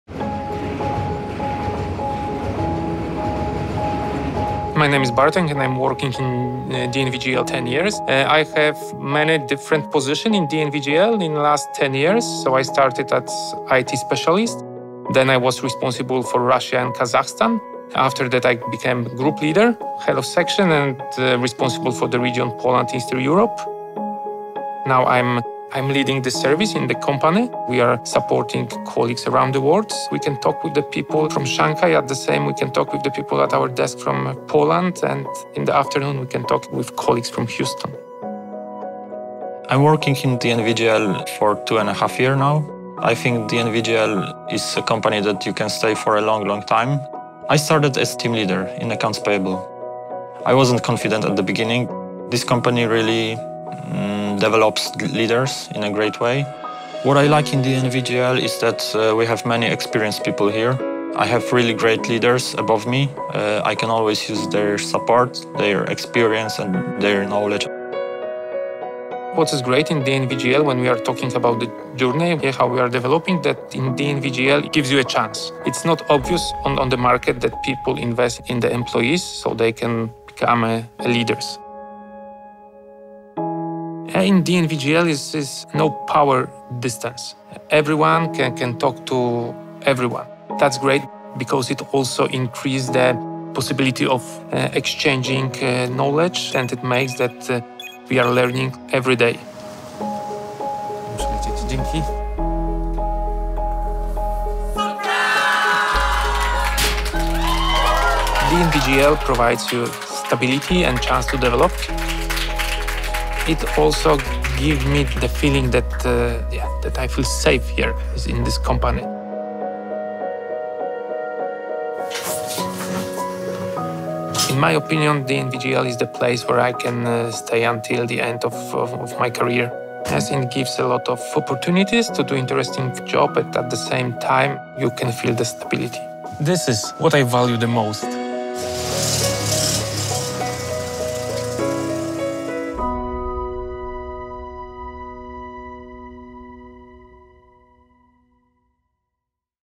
DNV GL Shared Services Gdynia - what Bartek and Patryk value the most (employee story)
Can career stability go hand in hand with personal development and a variety of challenges, where work is both stable and exciting? Of course it can!

Today, we want to introduce to you not one, but two of our employees!

Meet Bartek, an experienced IT manager, who started at DNV GL as a specialist over a decade ago. And Patryk, a Team Leader in Finance, who just recently has taken on the position of Head of Section.

Two different paths proving that DNV GL Shared Services is a place where talents are properly nurtured, and where the opportunities sprout providing for a lasting and interesting career.

Want to know more about DNV GL Shared Services?